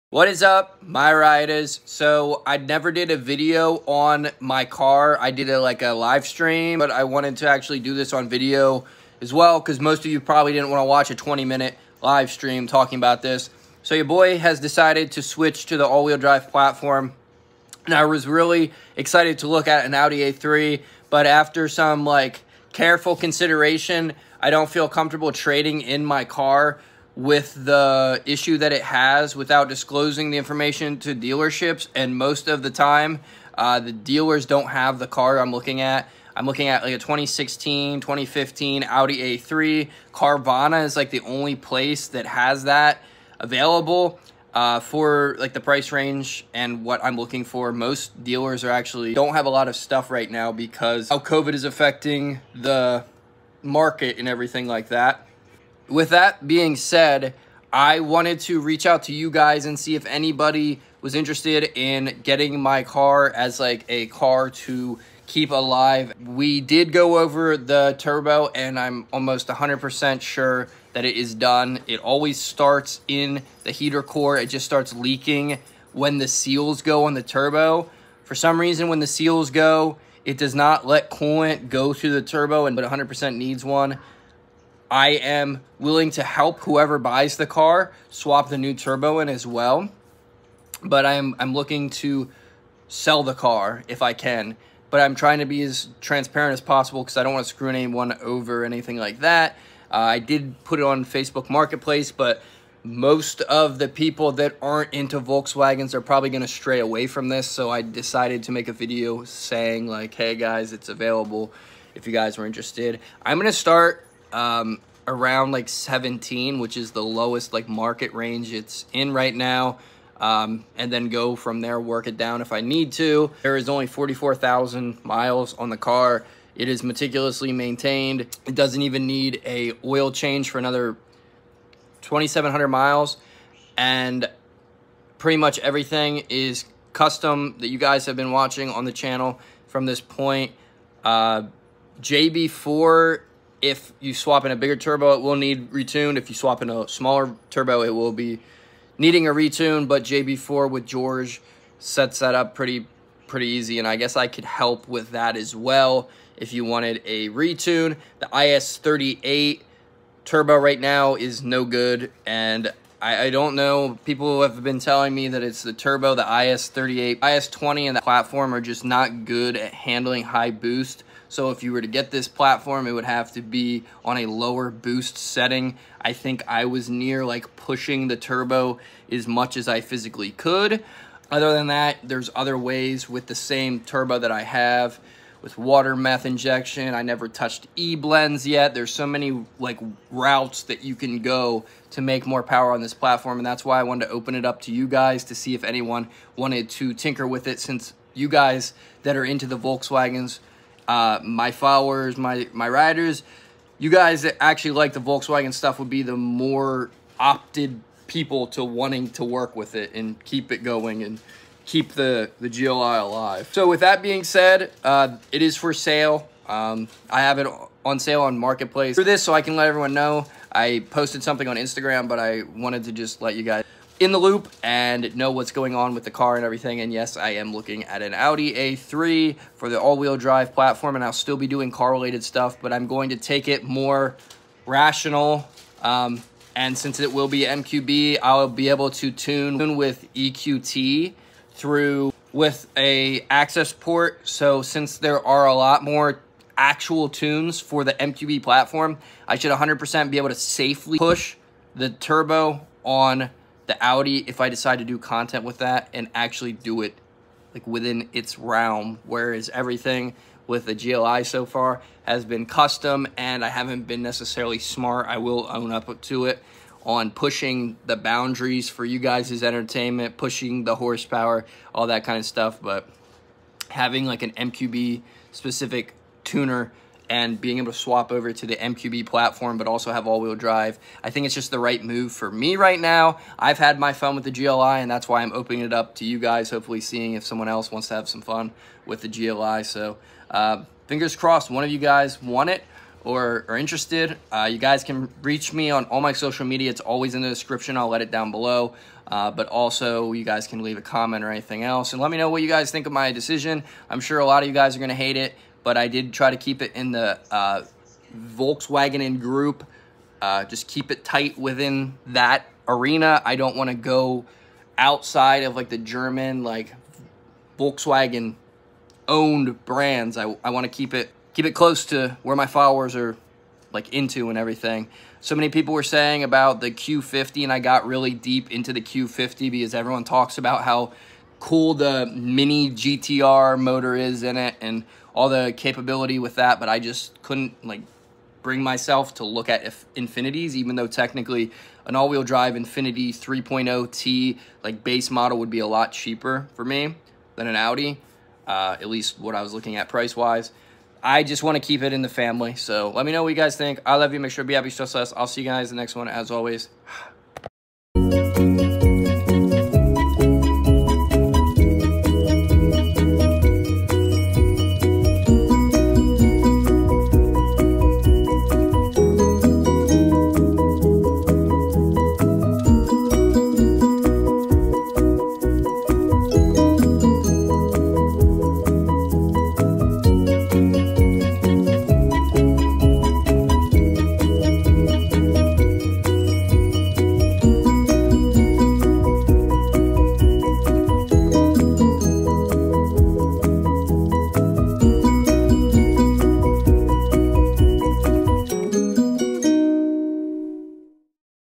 MK6 GLI IS38+ For Sale VW Subs Please Watch
Let me know what you guys think of my decision and if you would be interested. If you find a buyer I will give $50 to the referral person. Who wants to buy the GLI to keep it going and who is excited about the new platform?